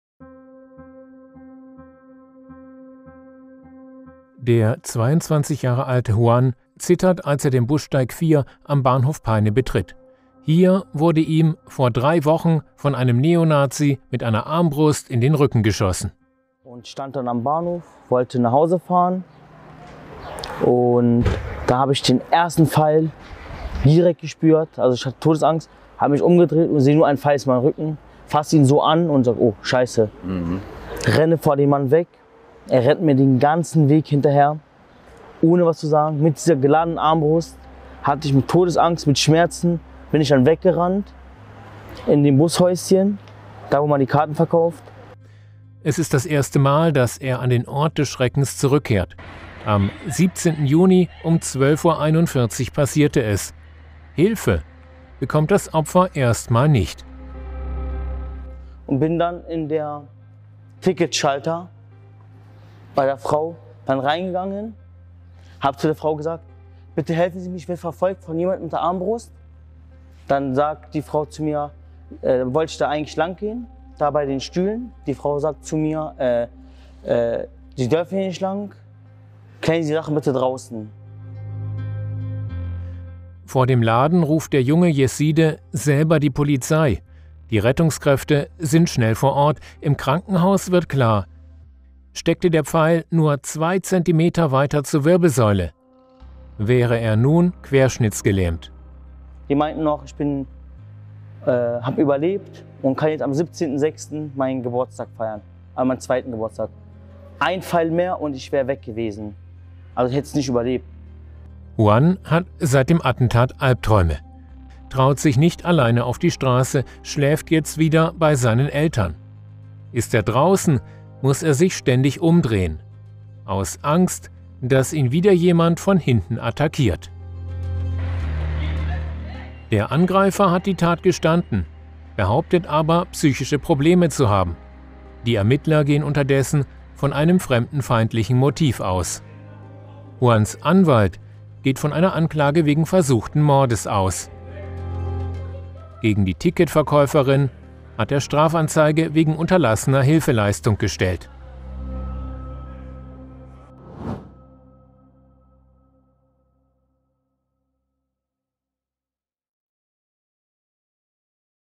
Ein Neonazi schoss ihm mit einer Armbrust in den Rücken
Als Juan Y. (22) auf den Bussteig 4 am Bahnhof Peine tritt, beginnt er zu zittern. Es ist das erste Mal, dass er an den Ort zurückkehrt, an dem er in Lebensgefahr schwebte.

Hier hat vor drei Wochen ein Neonazi mit seiner Armbrust dem jungen Jesiden in den Rücken geschossen!

Mit BILDplus lesen Sie, wie der junge Mann von dem Tag erzählt, der sein Leben veränderte und wie er nur mit Glück einer noch schlimmeren Katastrophe entging.